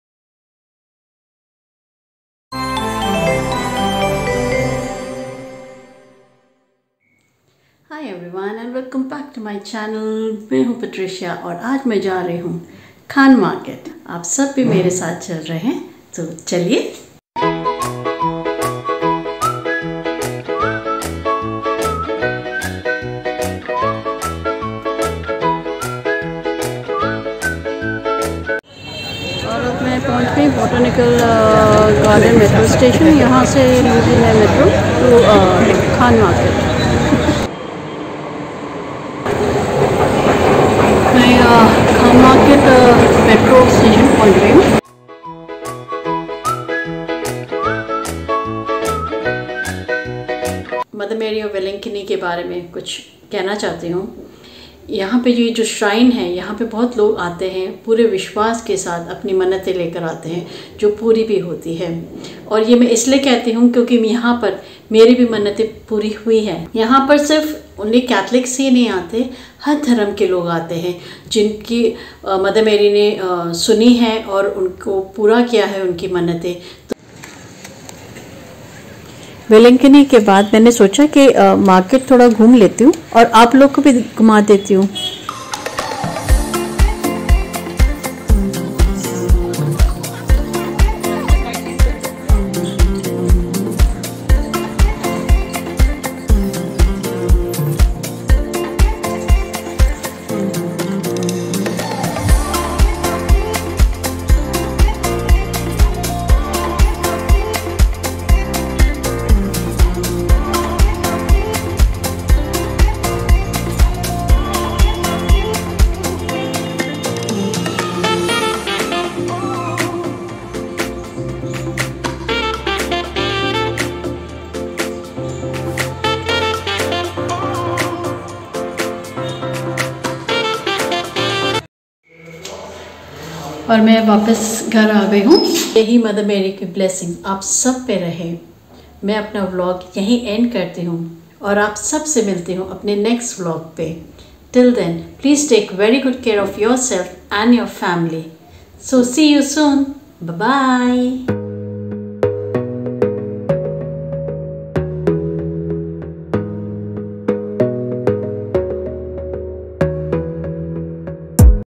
Went to Khan Market 😃
Visit to the Shrine of Mother Vailankanni in Khan Market.